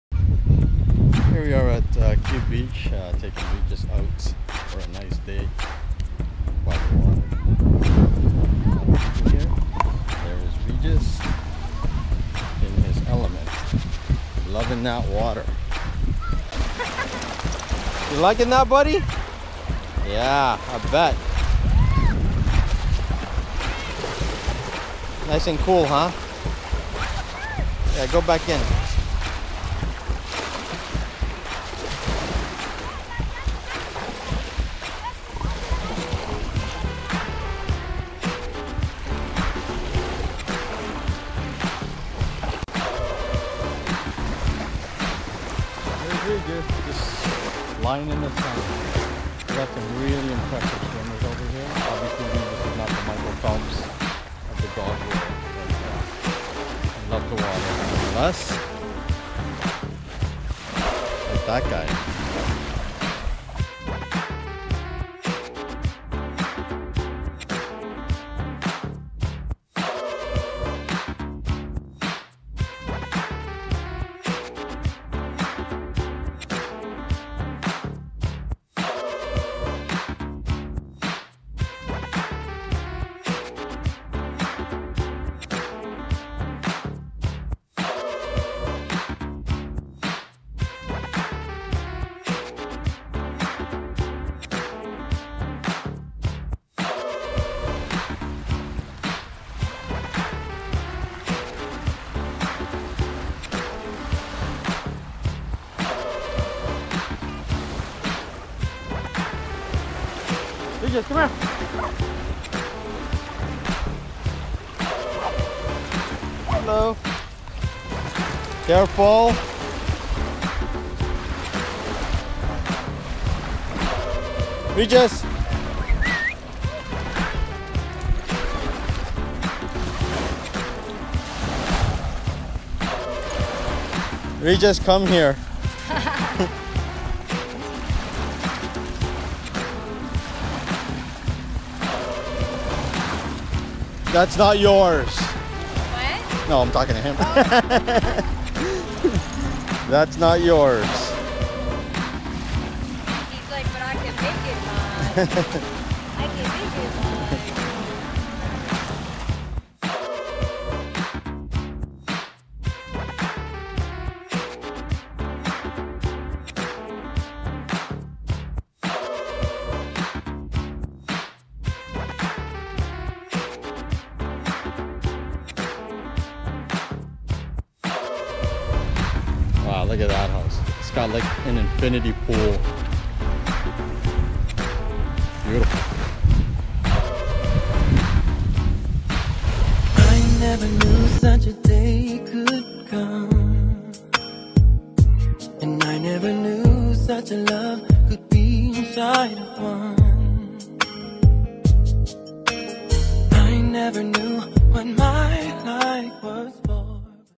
Atomic Regis Phelps
Regis goes to the beach for a swim and finds love.